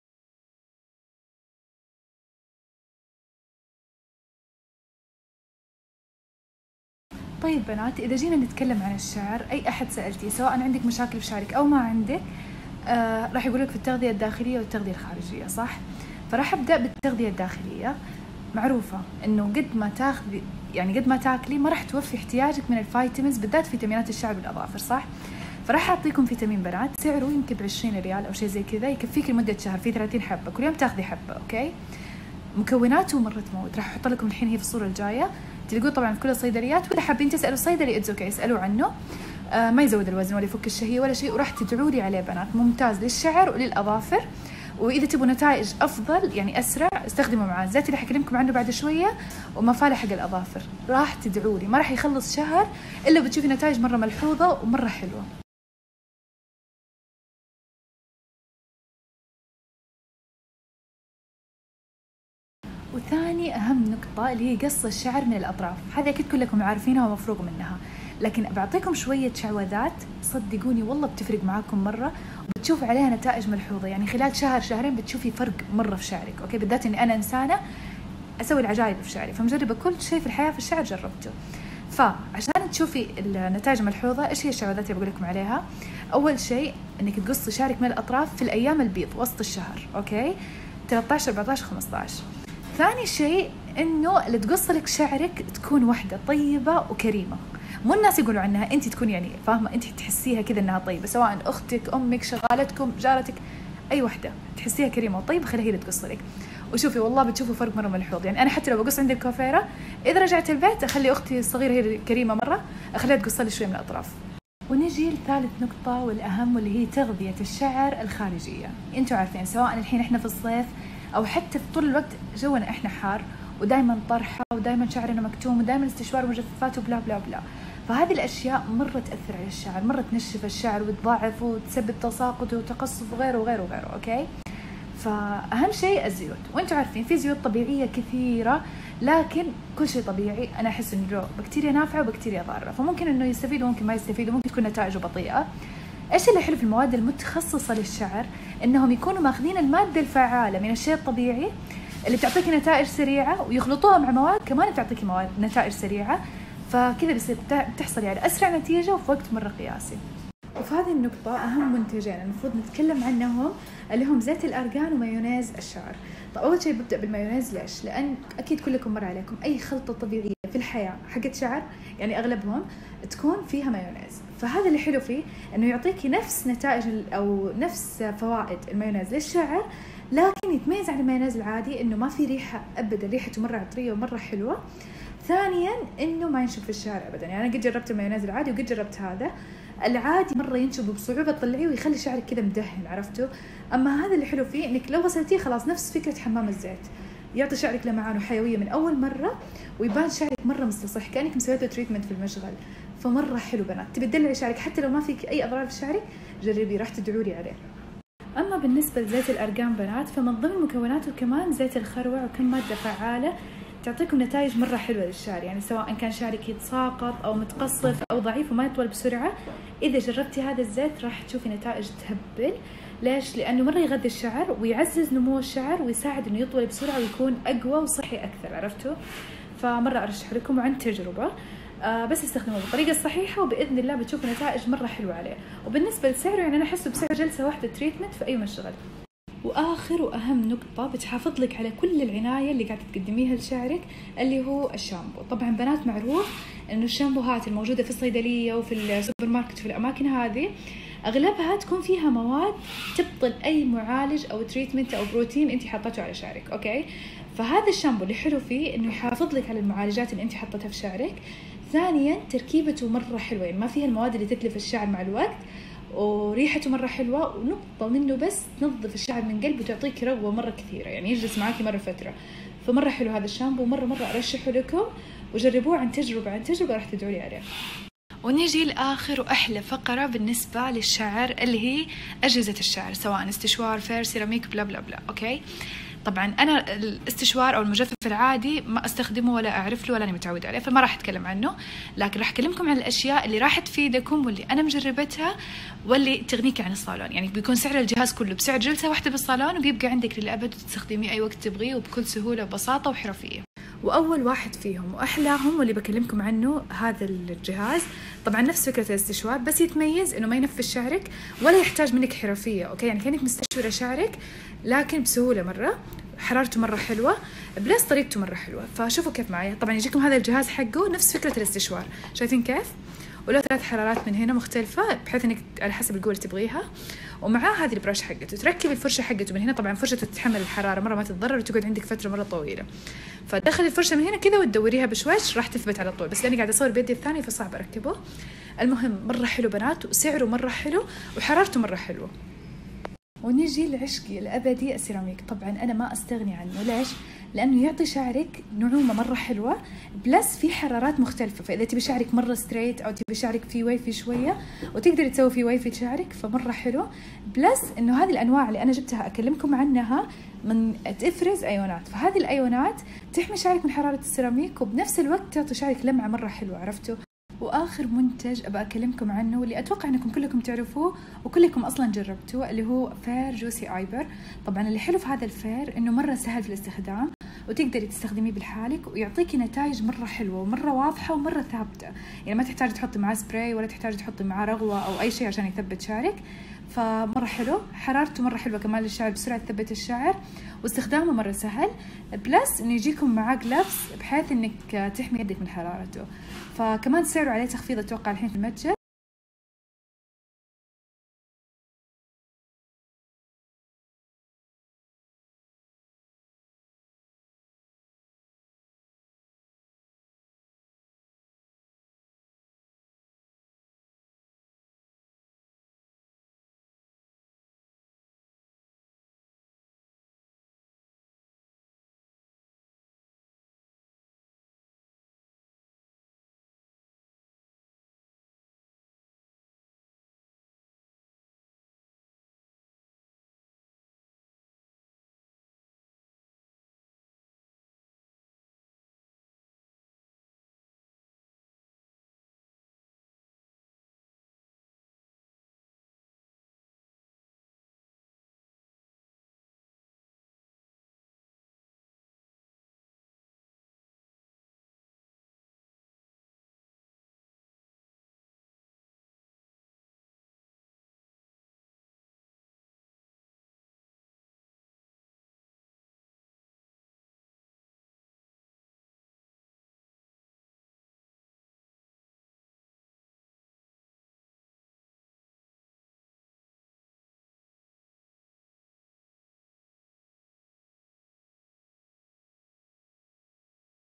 كل ما يخص الشعر(زيت،مكملات،مايونيز،خزعبلات، اجهزه)ونصيحة بيوتي ميلانو ✨
جميع المنتجات متوفره الاصليه منها في متجر بيوتي ميلانو

الفيتامينات المكمله مالها اي اضرار لأنها توفرلك احياجك اليومي فنسبتها معقوله
يمكنكم استشارة الطبيب او الصيدلي 💕

مقتطفات من سناب بيوتي ميلانو